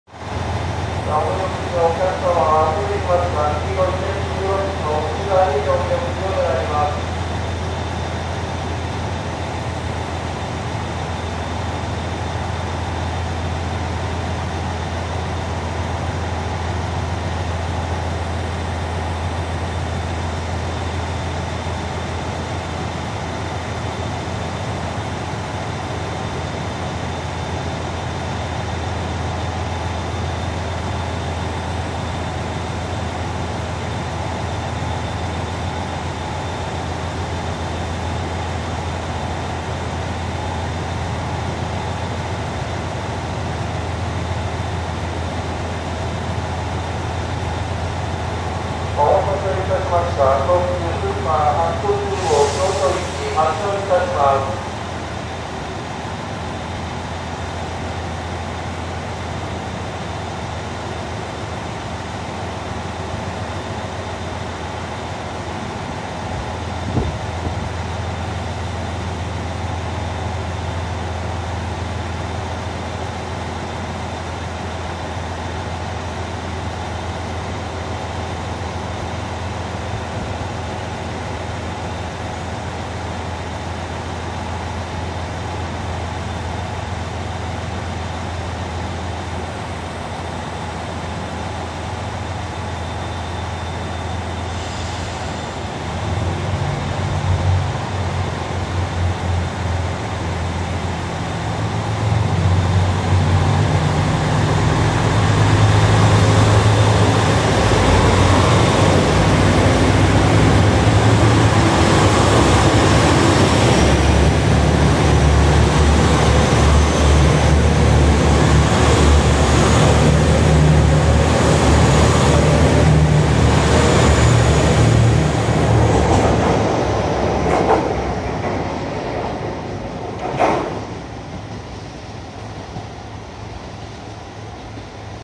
【お盆休み最終日】両非貫通編成 特急スーパーはくと10号 京都行き 鳥取駅発車
お盆休み最終日となり 多くの人が乗車していました
入り口に立っている人の姿も見られました